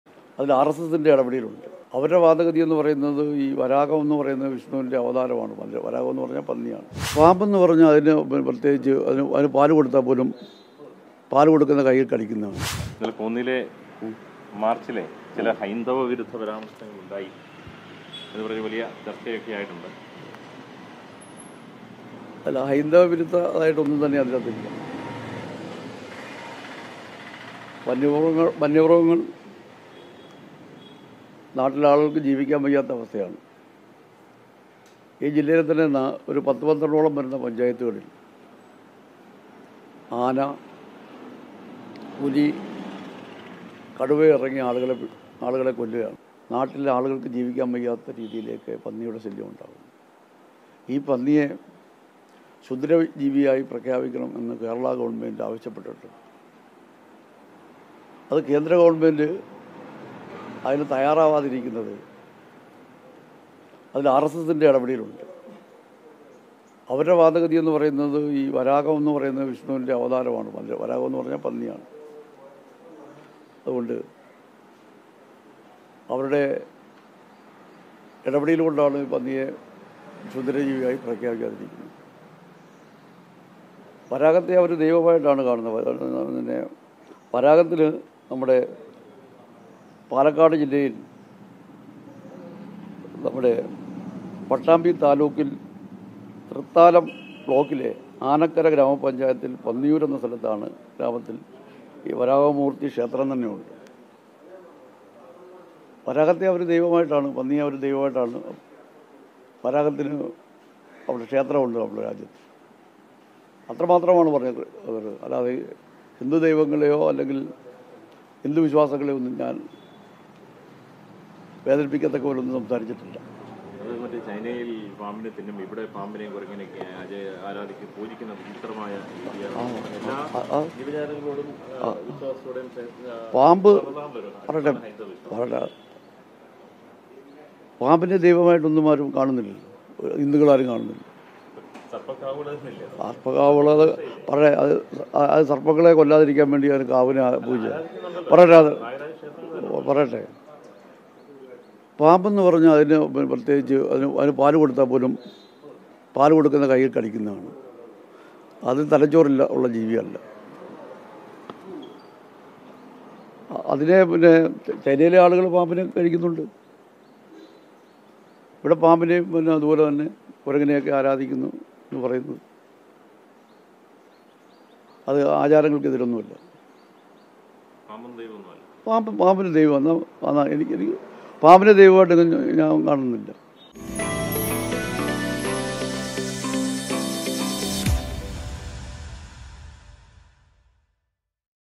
ആരോപണങ്ങള്‍ക്ക് മറുപടിയുമായി സിപിഎം പത്തനംതിട്ട ജില്ലാ സെക്രട്ടറി കെ പി ഉദയഭാനു | K. P. Udayabhanu
ഹൈന്ദവവിശ്വാസങ്ങളെ അപമാനിച്ചു എന്ന ആരോപണങ്ങള്‍ക്ക് മറുപടിയുമായി സിപിഎം പത്തനംതിട്ട ജില്ലാ സെക്രട്ടറി കെ പി  ഉദയഭാനു. കൃഷി നശിപ്പിക്കുന്ന കാട്ടുപന്നിയെ കേന്ദ്ര സര്‍ക്കാര്‍ ക്ഷുദ്ര ജീവിയായി പ്രഖ്യാപിക്കാത്തതിന് പിന്നില്‍ ആര്‍.എസ്.എസ്. പന്നി മഹാവിഷ്ണുവിന്‍റെ അവതാരായ വരാഹമെന്ന വിശ്വാസമാണ് കാരണമെന്നും ഉദയഭാനു